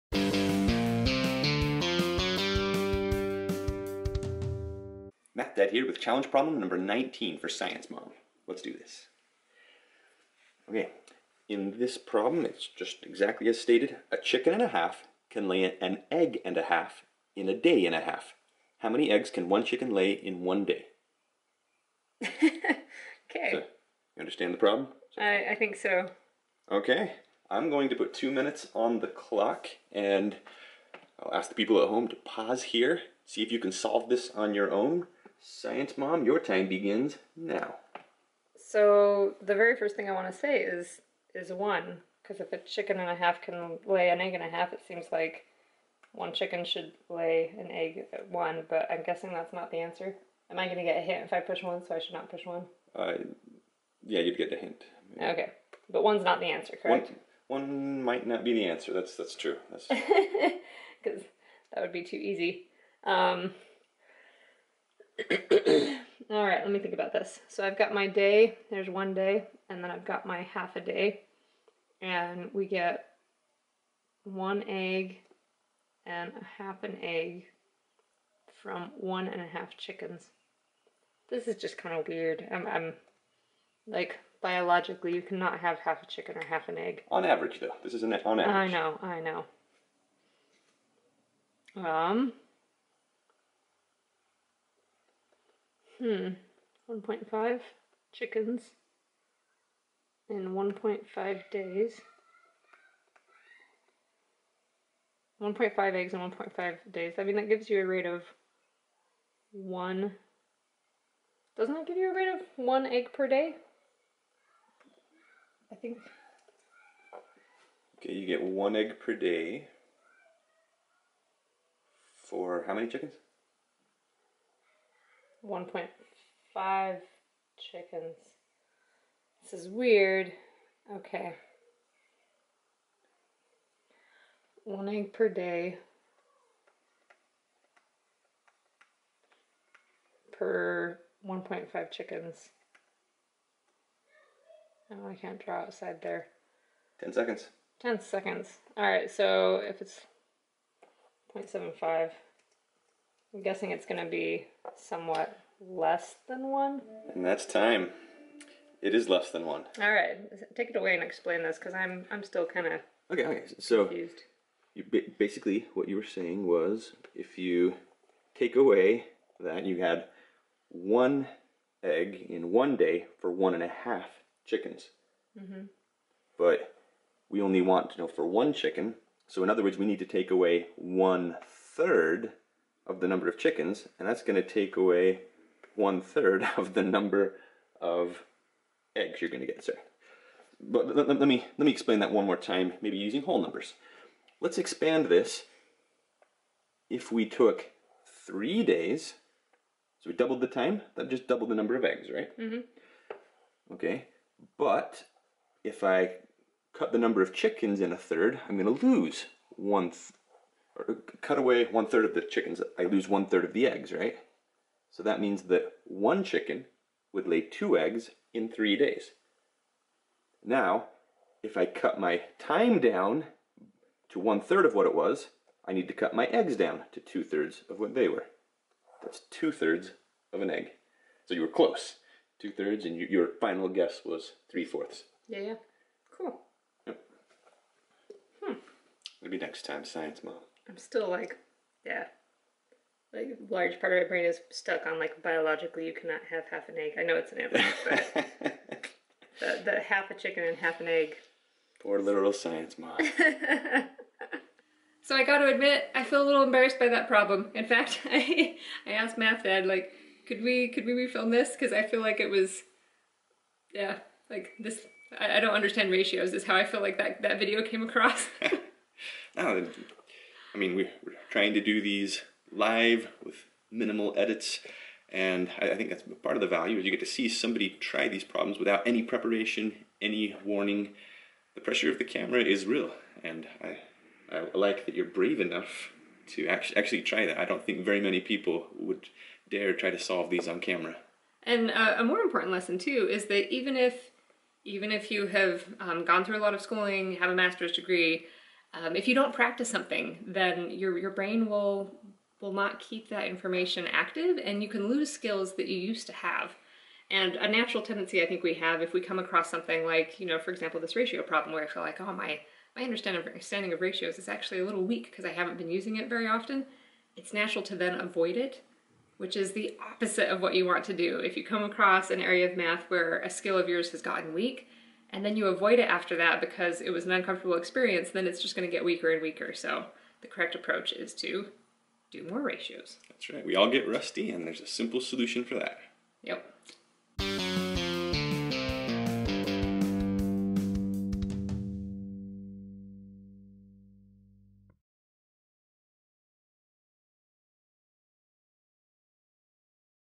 Math Dad vs Science Mom 19
If a chicken and a half can lay an egg and a half in a day and a half, how many eggs can one chicken lay in one day? Good luck Science Mom.